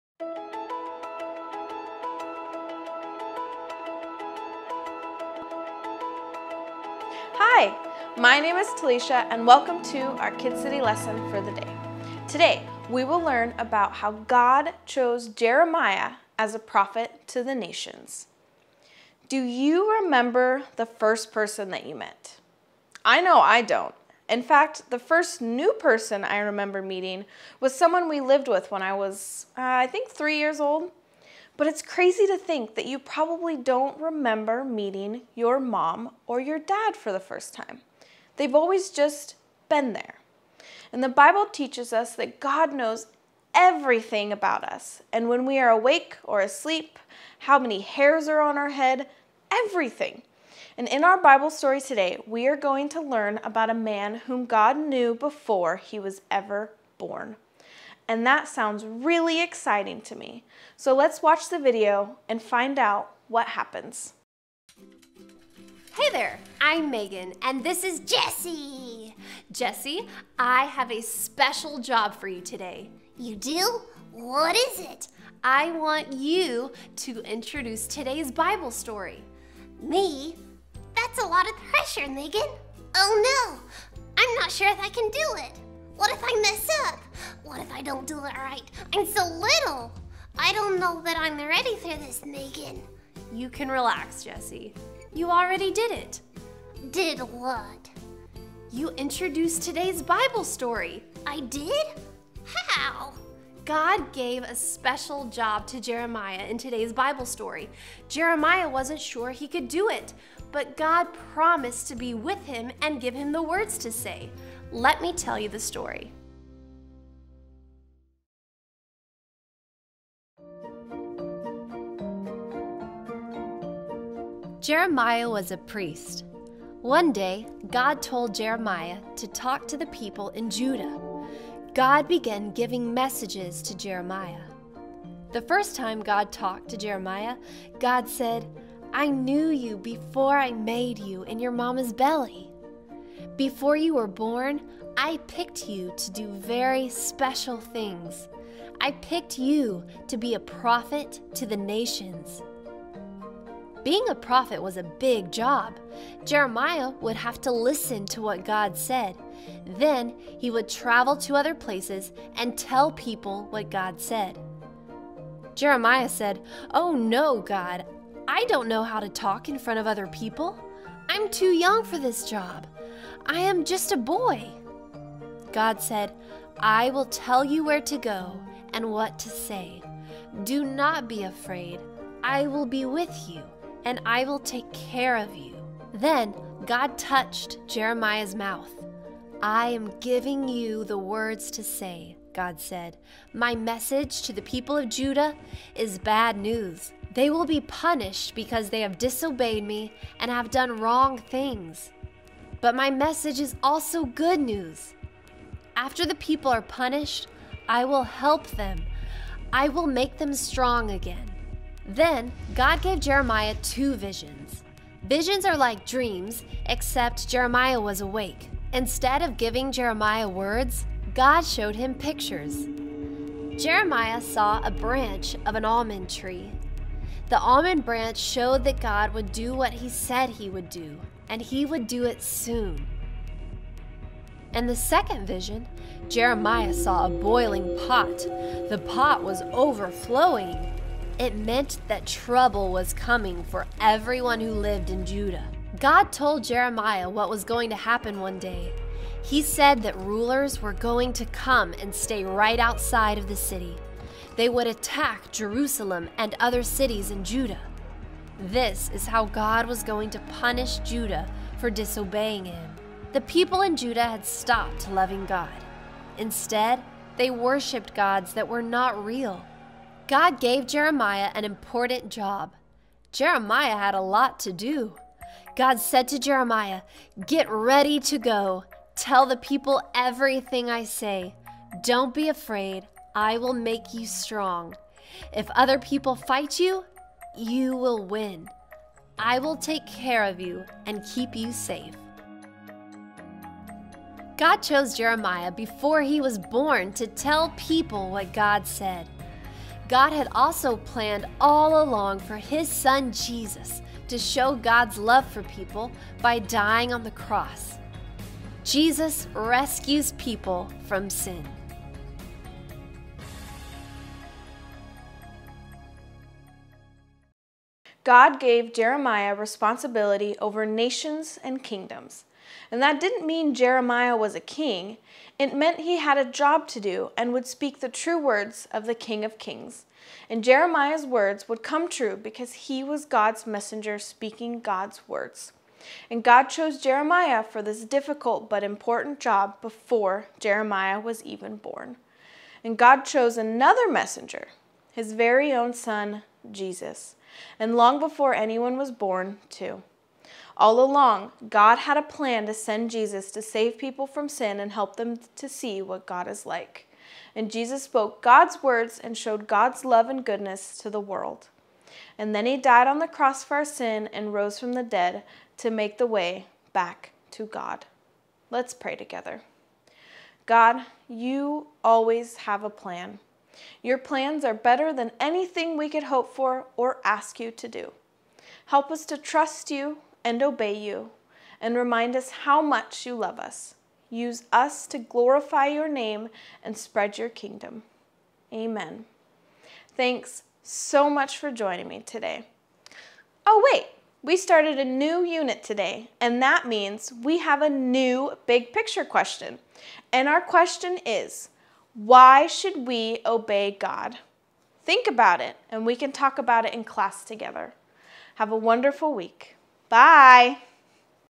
Kid City Online | September 4, 2022
Telisha is back with another Kid City lesson and this week we are talking about how God called Jeremiah.